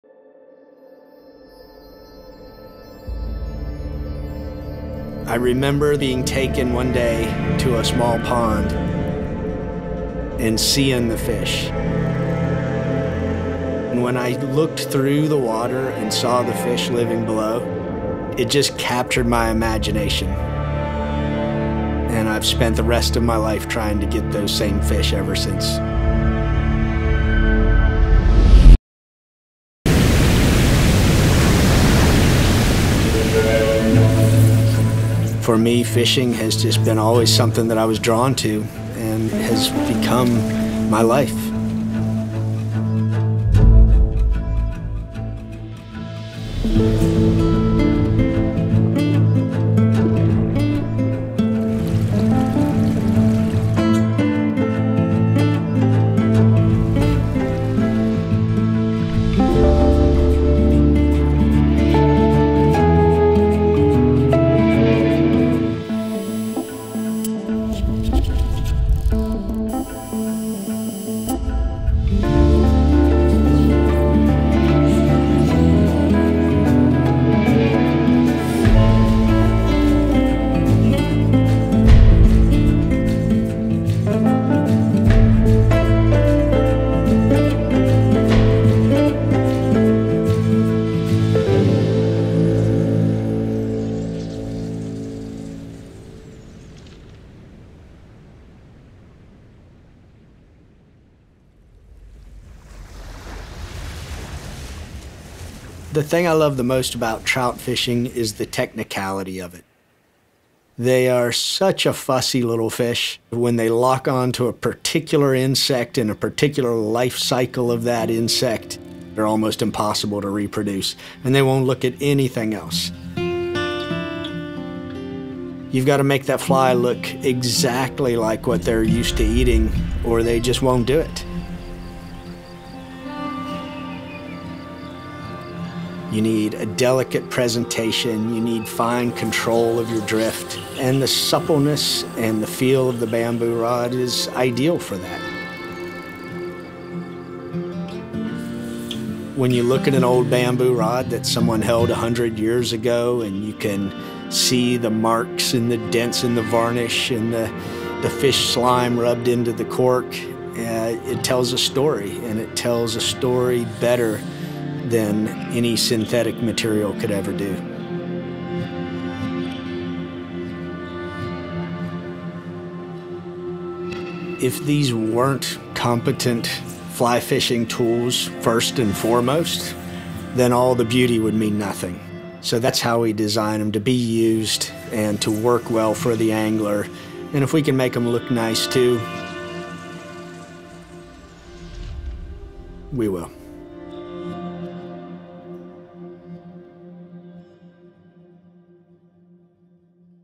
River Fly Fishing for Trout with Bill Oyster and Oyster Bamboo Fly Rods
"I remember being taken one day to a small pond and seeing the fish, and when I looked through the water and saw the fish living below, it just captured my imagination, and I've spent the rest of my life trying to get those same fish ever since."  - Bill Oyster

The size and depth of large streams can vary, offering a range of fishing opportunities from shallow riffles to deep pools and runs. Oyster Bamboo's expertise in crafting bamboo fly rods allow us to tailor the design to best suit the angler's need and fishing situation at hand.

Cinematographer/Editor/Colorist Shane Miller
shanemiller.co
Audio Post production by Fellow Wave